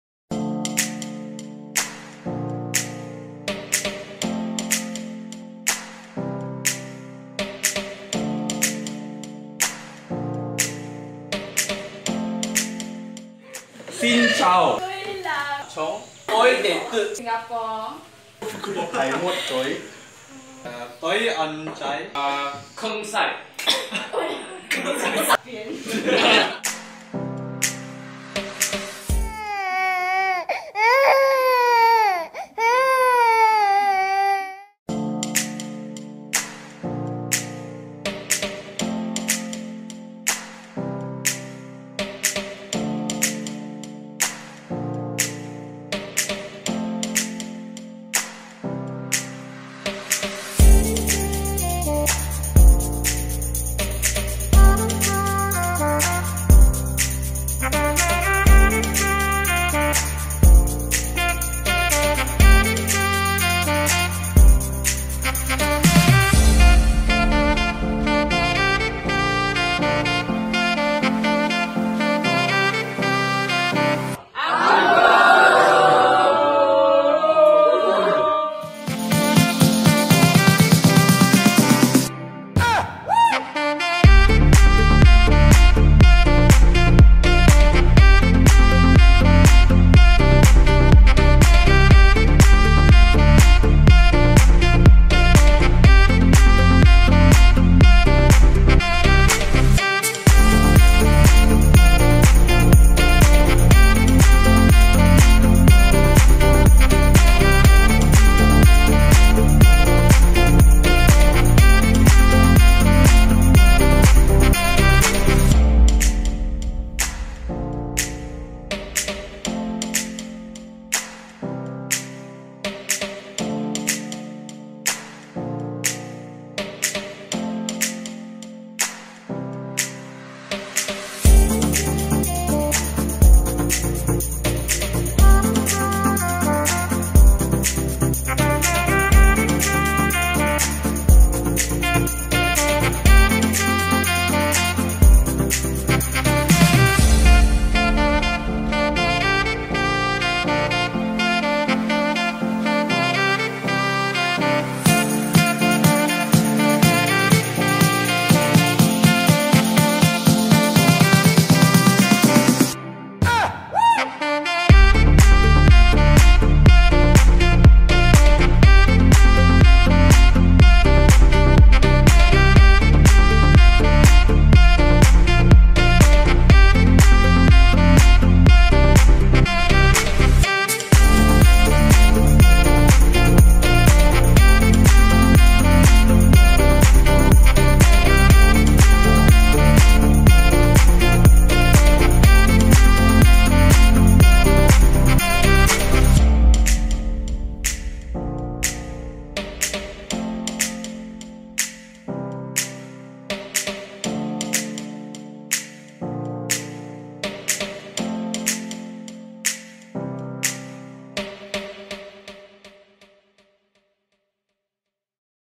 YESD - NUS PROJECT ANGEL XXII - Volunteering in Nam Te village, Phuong Tien, Vi Xuyen, Ha Giang
It has been 20 days with tears and sweat but finally we made it! This is what we are proud of.
What did we do?
1. Build a new small kindergarten for the local kids up in the mountain of Nam Te village
2. Organize cultural exchange with local people in the villages
3. Organize the sport day, game day and other educational activities for the kids in the village and in the orphanage

Thanks all members of Project Angel XXII to make it possible